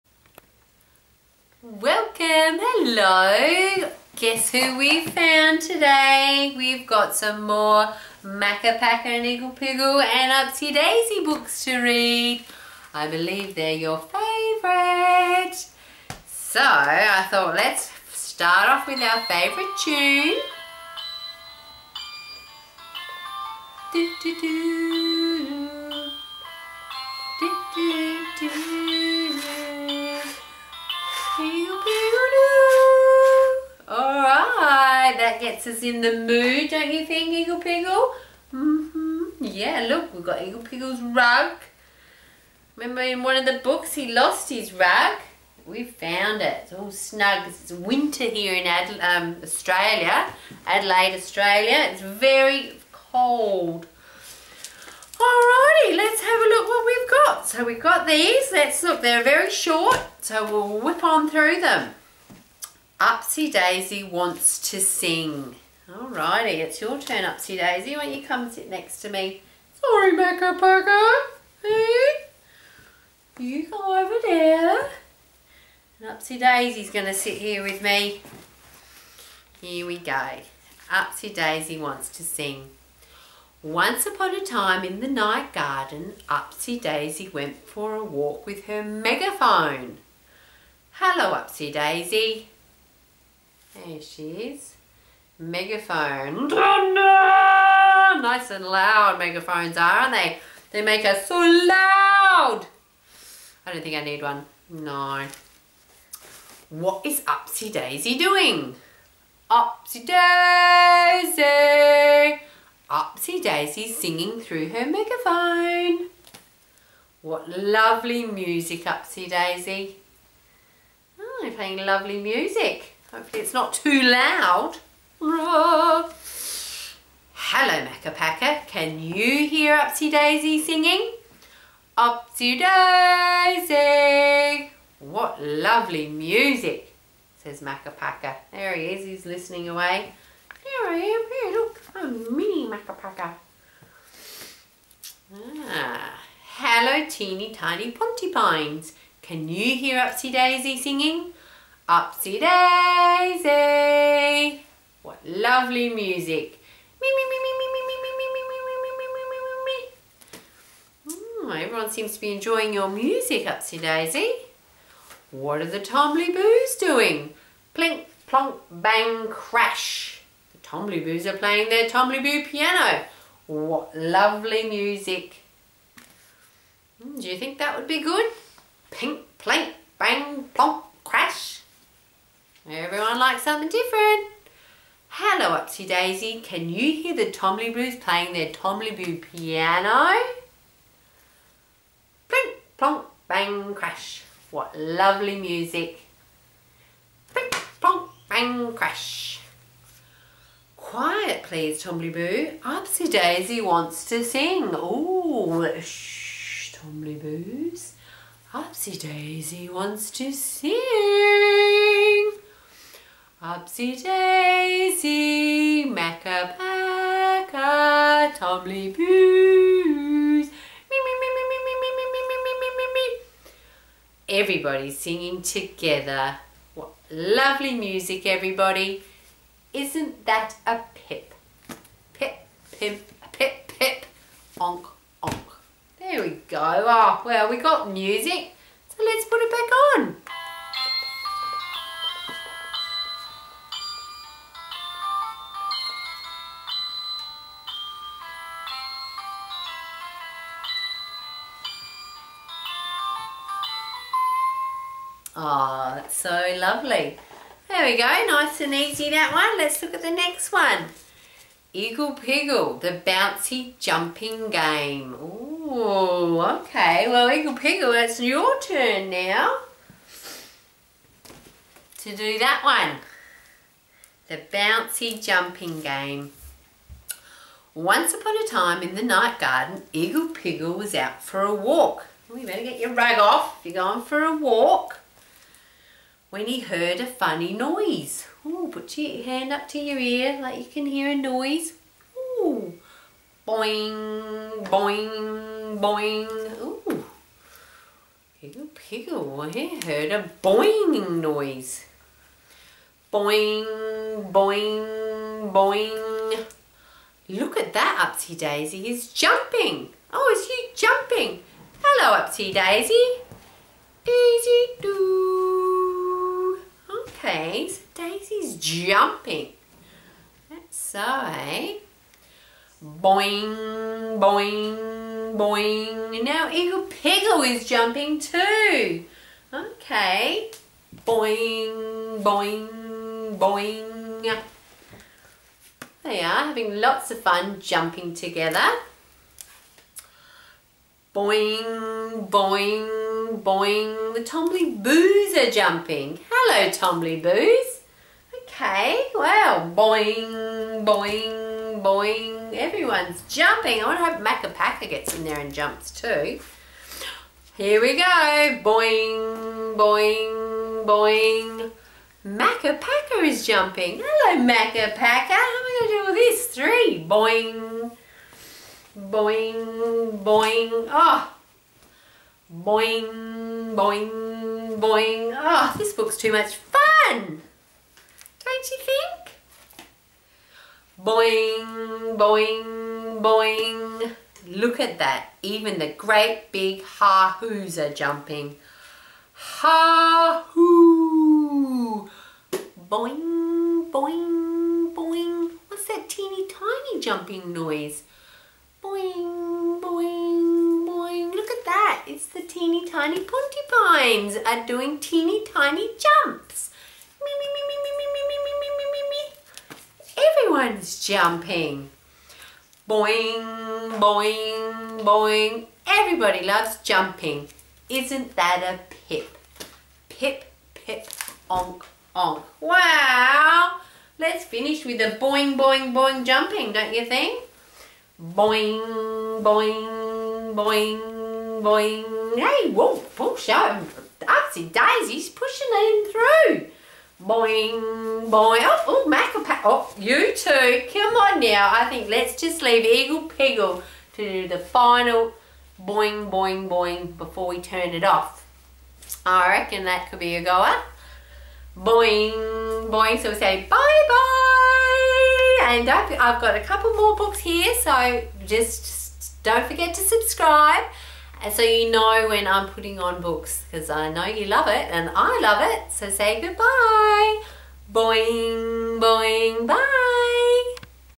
Kids Books Read Aloud 💕 In the Night Garden by Andrew Davenport, read by Catherine Elliott
Its time to read two In the Night Garden Books, Upsy Daisy wants to sing! and The Bouncy Jumping Game. Sit back and enjoy the fun and see what mischief we get up too..